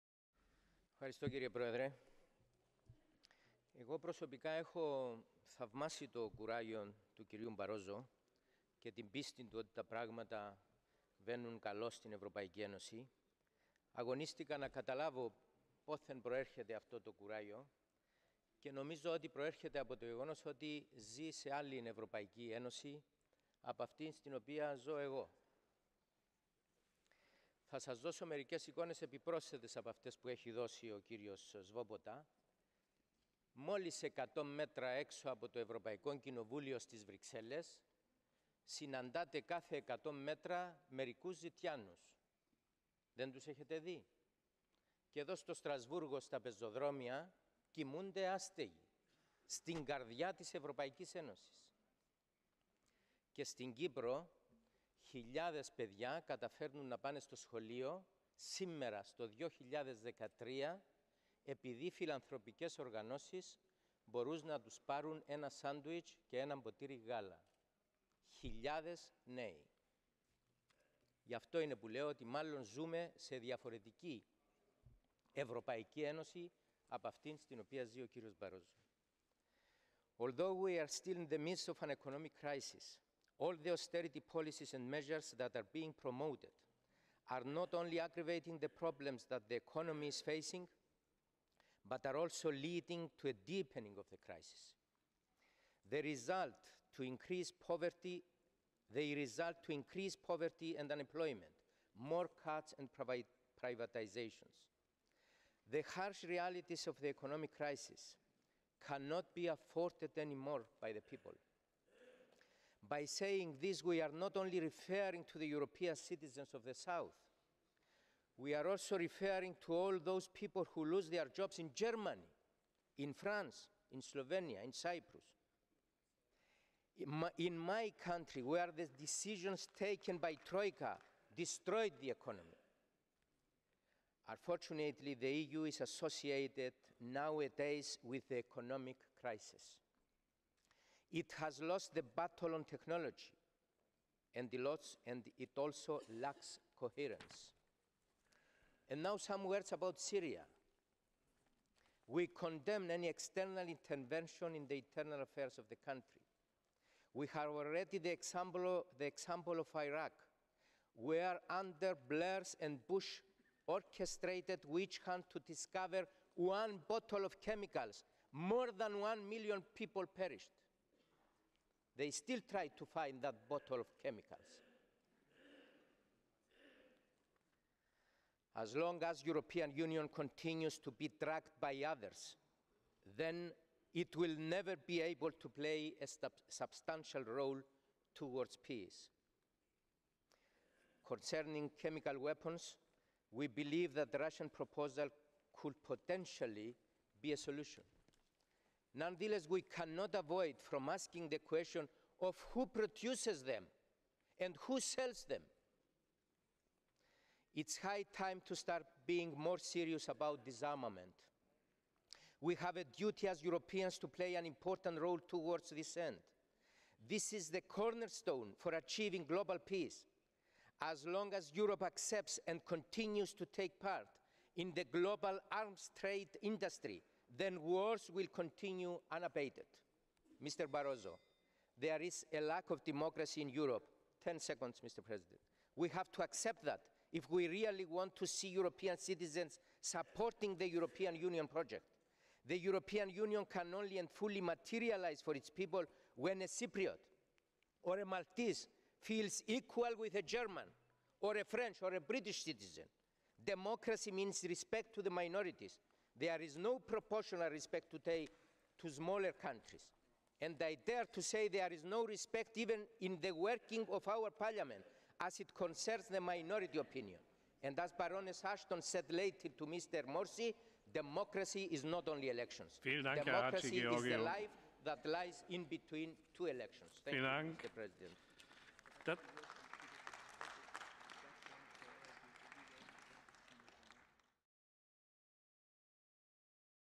Takis Hadjigeorgiou @ SOTEU 2013
Takis Hadjigeorgiou, vice-chair of Parliament's GUE/NGL group, responded to Commission president Barroso's State of the Union address.